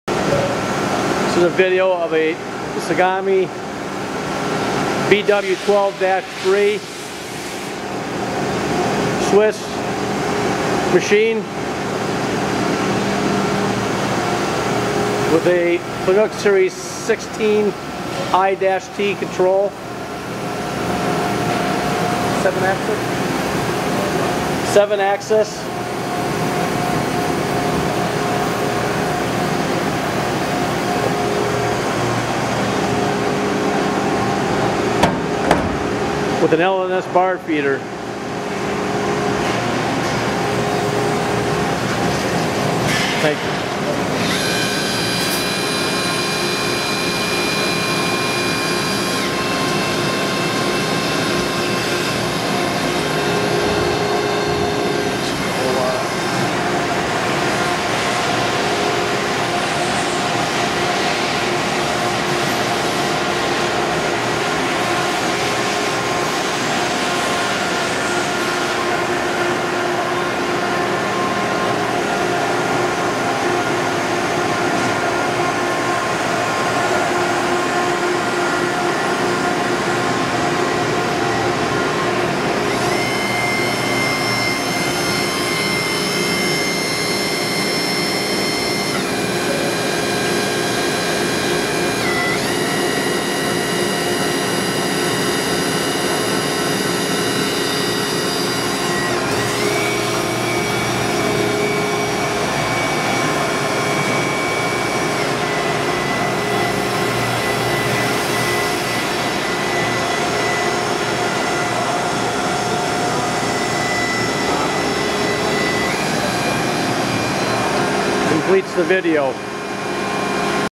2007 TSUGAMI Model BW12-III CNC 7 Axis Swiss Type Screw Machine with Fanuc Ser. 16i-T Control.
2007 TSUGAMI Model BW12-III CNC 7 Axis Swiss Type Automatic Screw Machine with Fanuc Series 16i-T Control, With Back Spindle, Controllable Axes 7 (X1, Y1, Z1, X2, Y2, Z2, X3, Y3), Total Tools 21, (Live Cross Drills: 5, OD Turning: 7, ID Tools: 5, Back Work Tools: 4), Main and Back Spindle Indexing, Max. Dia. 12mm, Max. Drilling Dia. 7mm, Max. Tapping Dia. M6 x 1, Max. Cross Drilling Dia. 6mm, Max. Cross Tapping Dia. M5 x 0.8, Spindle Speeds 200 – 12,000 RPM’s, LNS 3.10 HS-4.8 Bar Feed, Coolant Tank and Pump, Tri Mist Collector. Mach. T2.